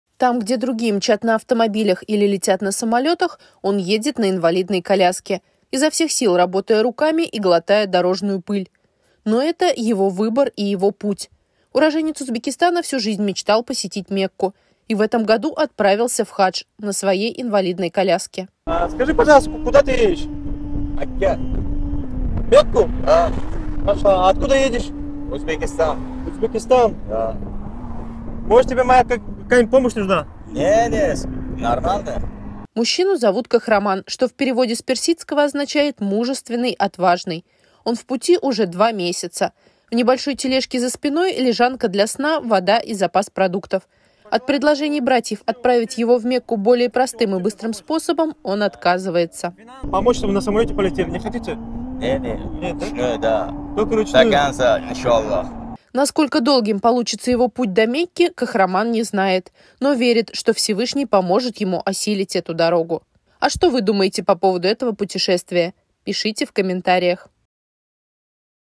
Узбек отправился в Мекку на инвалидной коляске!
Там, где другие мчат на автомобилях или летят на самолетах, он едет на инвалидной коляске. Изо всех сил работая руками и глотая дорожную пыль. Но это – его выбор и его путь!